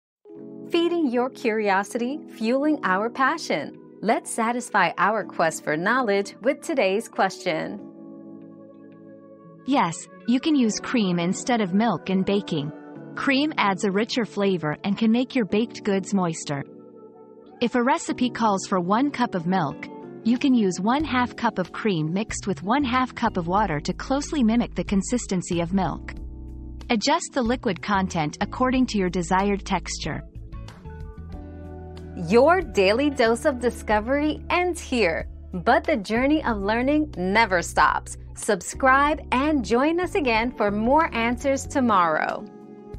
Can you use cream instead of milk in baking?
Cream vs Milk in Baking: What You Need to Know 👉 Creamy Baking Secrets 👉 Discover how using cream instead of milk in baking can elevate the flavor and moisture of your treats. Learn how to substitute cream for milk in your recipes for delicious results.

Laura S. Harris (2024, June 24.) Can you use cream instead of milk in baking?
    🌐 AskAbout.video